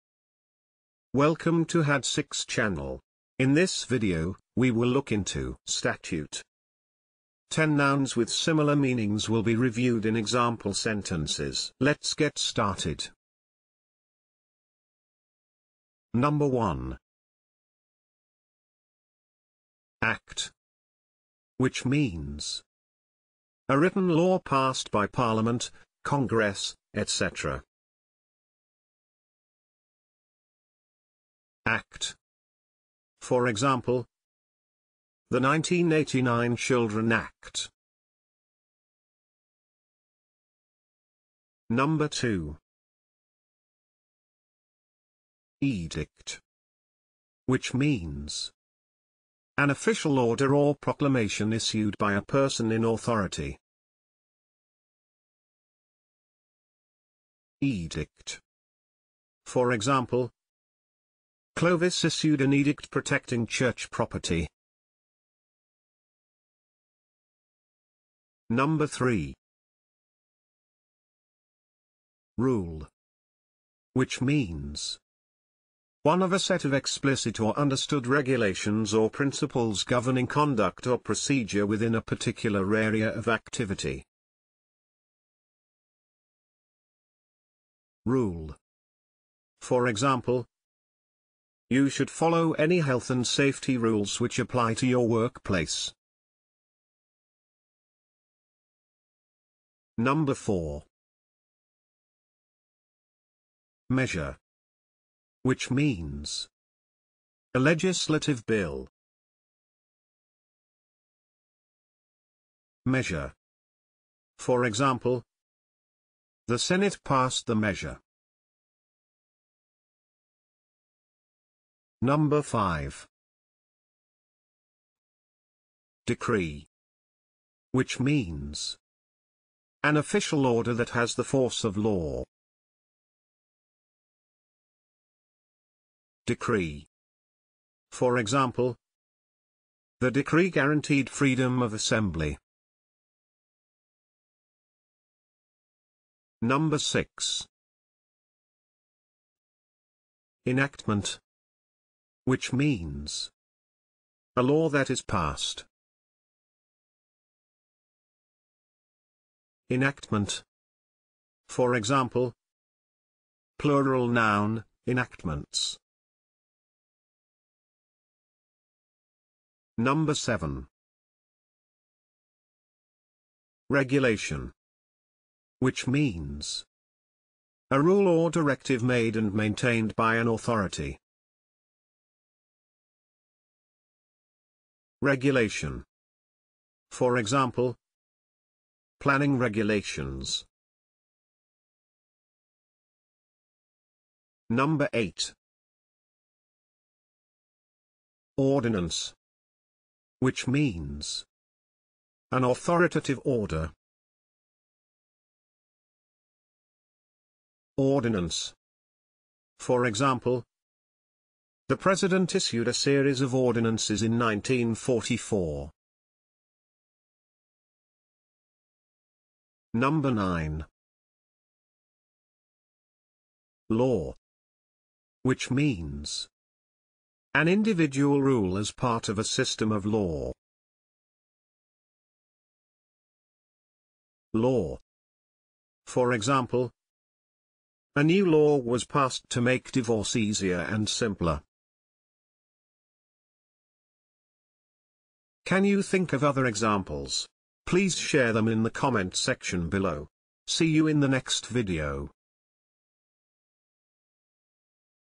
statute - 10 nouns which are synonym to statute (sentence examples)
This video examines statute as a noun. Here, 10 nouns, which have meanings similar to that of statute in standard English, are introduced through sentence examples. These synonyms help you to remember the meanings of this group of words for improving your vocabulary.